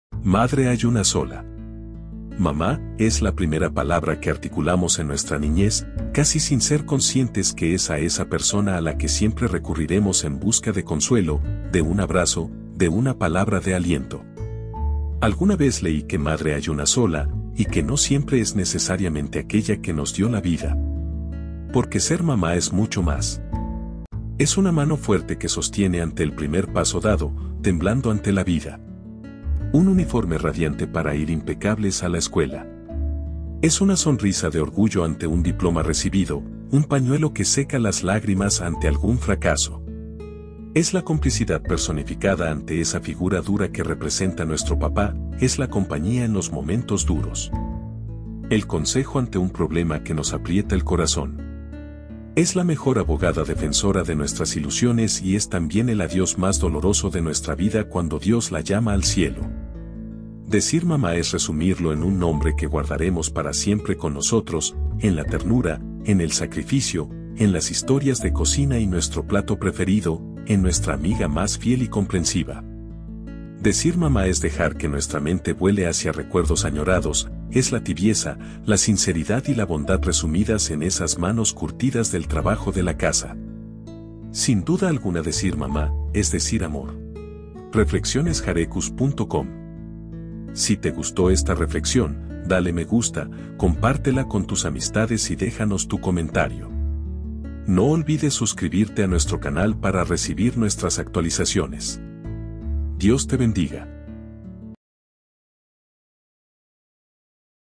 Madre hay una sola. - Reflexiones para mamá
👩‍👧‍👦💖 "Madre hay una sola"
- Un homenaje al amor incondicional y al papel único de las madres en nuestras vidas. Exploramos cómo ser mamá va más allá de la sangre, es un vínculo lleno de abrazos, consejos y apoyo. 🌼✨

Descubre cómo la figura materna nos acompaña en cada paso de la vida, desde los primeros pasos hasta los momentos más difíciles. Exploramos la importancia de una madre, sus consejos, su apoyo y su amor inquebrantable.

📚 En este video, reflexionamos sobre lo que significa ser mamá, cómo su influencia nos moldea y cómo su amor trasciende todas las fronteras. Un recordatorio de que decir "mamá" es decir amor en su forma más pura y genuina. 💐🌟

¡No te pierdas esta emotiva reflexión sobre la figura materna y su impacto en nuestras vidas!